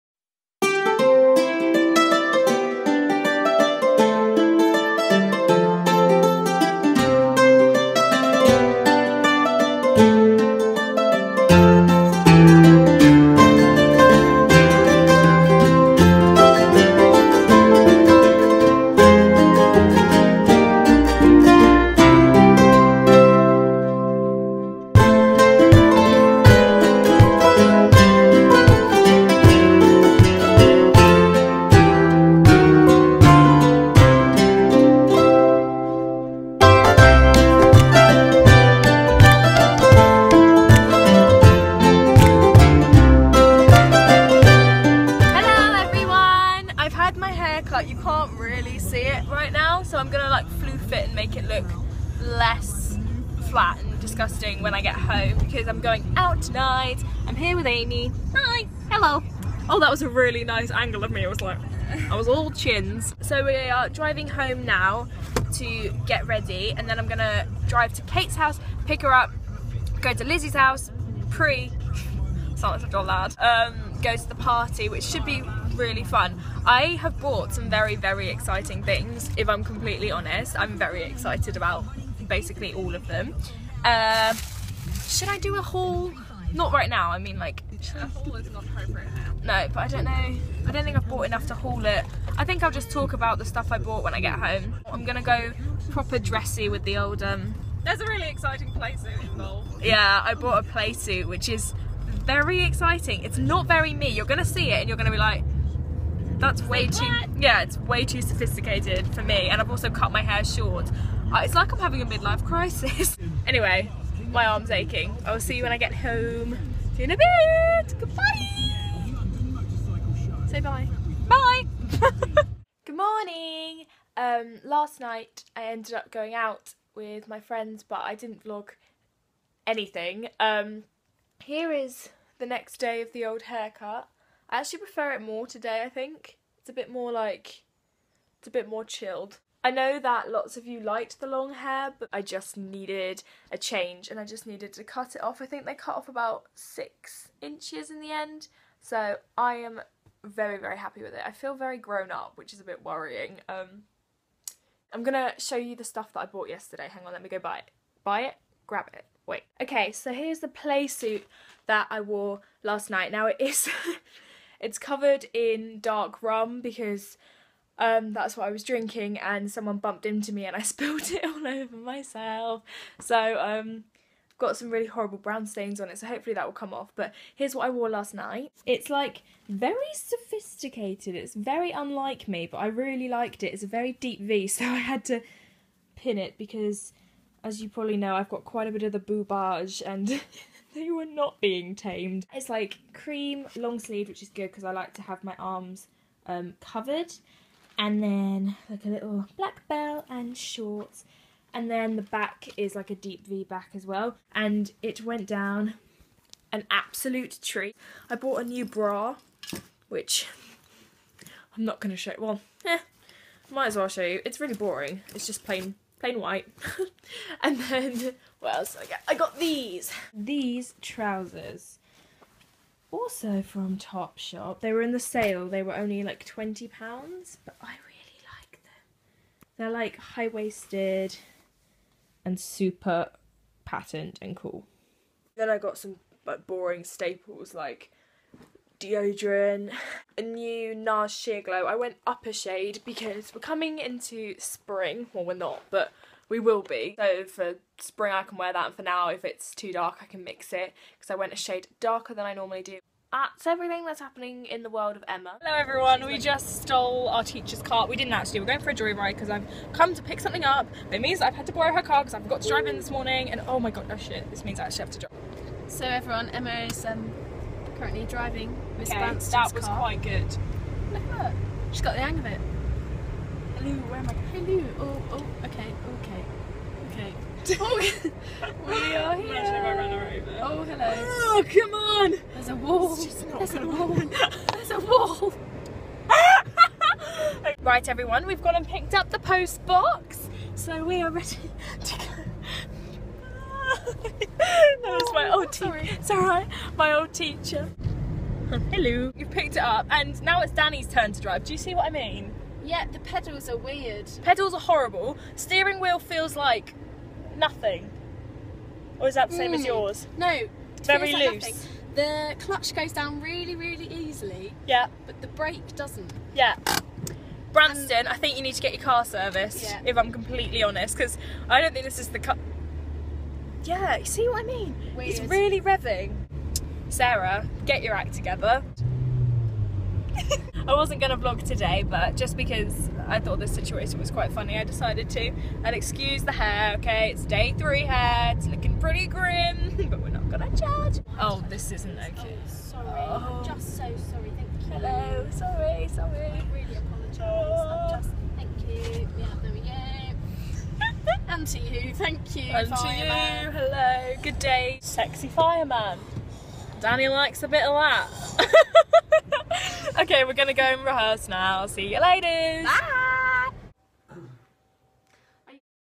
Weekly VLOG #12 | I cut my hair and go joy riding. | EmmasRectangle
I am sorry about how crappy I have been at filming/ uploading recently... I have had a crazy few weeks and hope to get back into the swing of things ASAP.. Here is a mini vlog just to show you I am alive :)